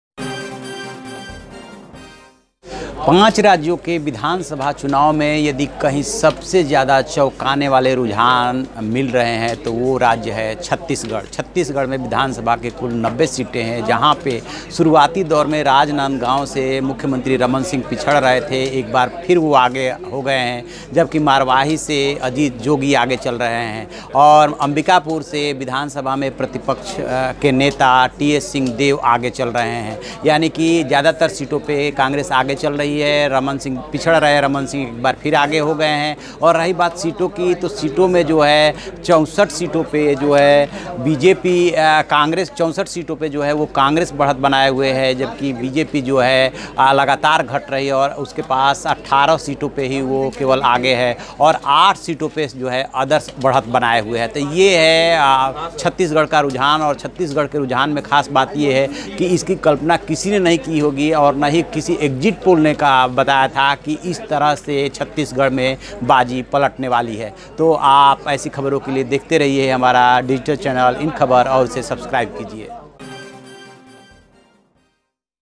Chhattisgarh Election Result 2018: Counting till 11:30 AM
Counting of votes for assembly elections 2018 is underway. Exit polls suggested a tough competition between Congress and BJP in Chhattisgarh. Although it is not a final result, the situation will be cleared after the final result of 2018.